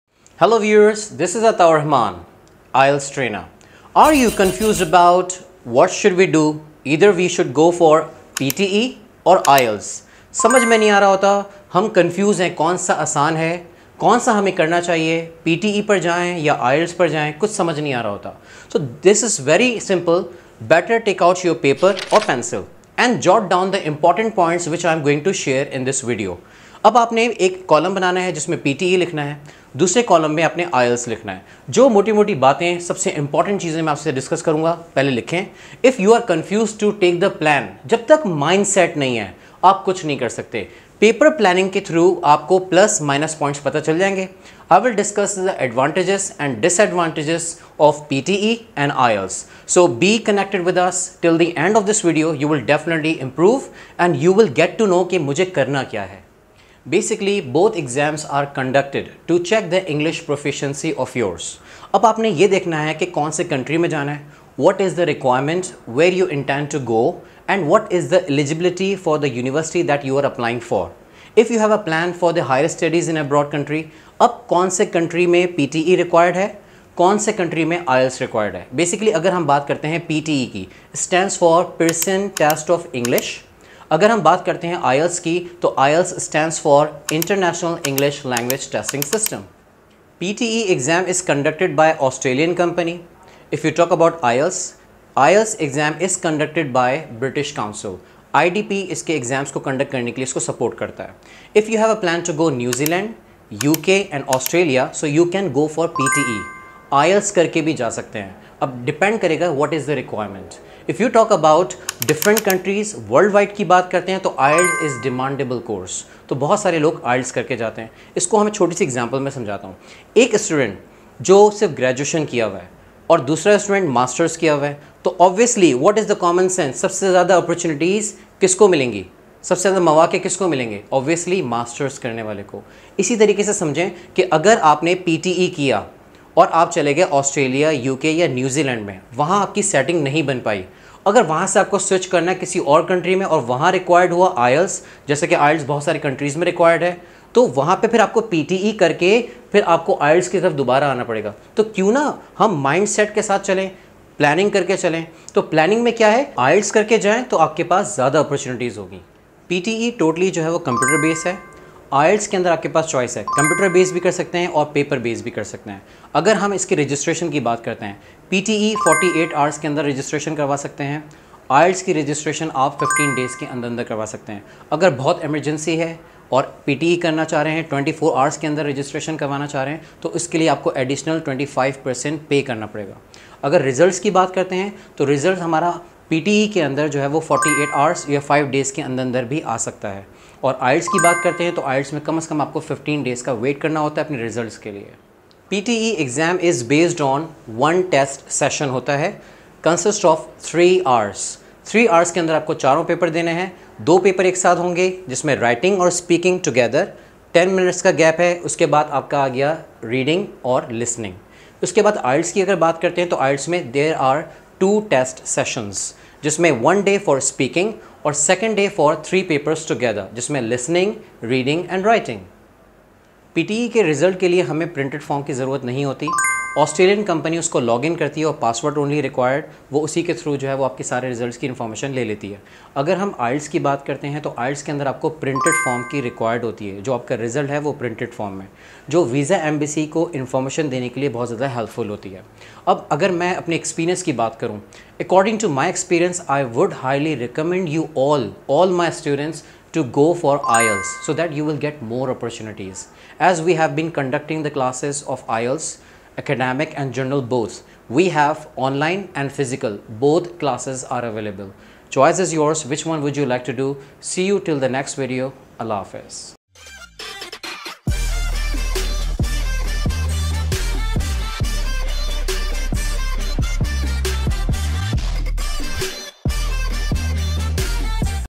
IELTS or PTE? Which is Better for You? | Pak College
Many students remain confused as to what to do, IELTS or PTA?
To know the best answer, watch this video and eradicate your confusion.